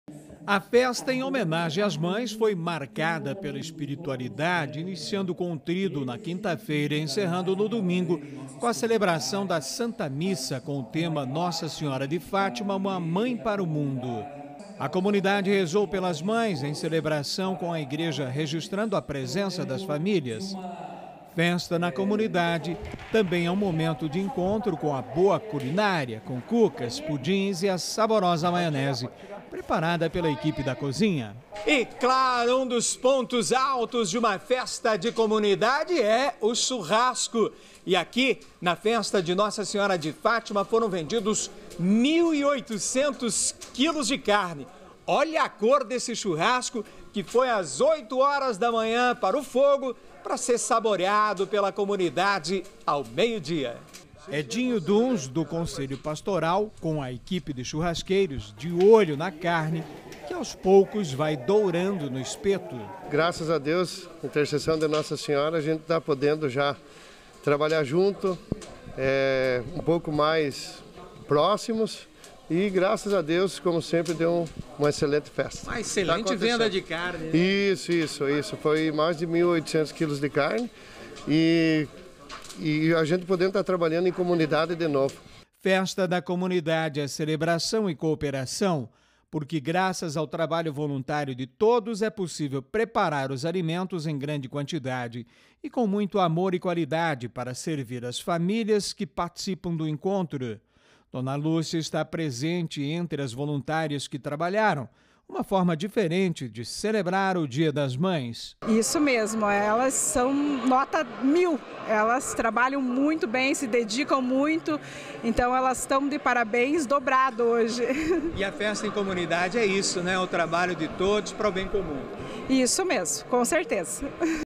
Comunidade da Paróquia Nossa Senhora de Fátima celebra em festa
Religião: A Paróquia da Nossa Senhora de Fátima homenageou a padroeira e as mães com festa.